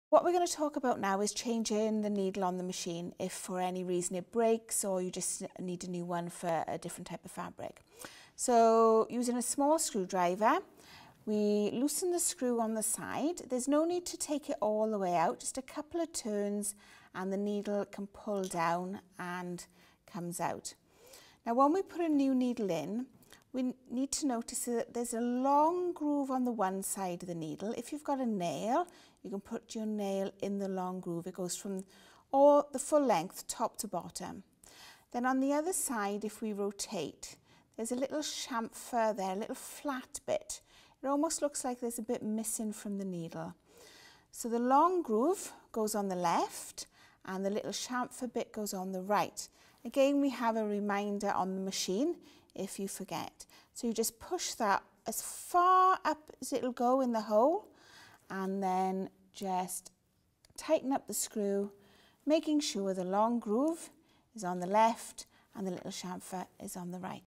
Lockstitch - changing a needle
This video explains how to change a needle on an industrial lockstitch sewing machine.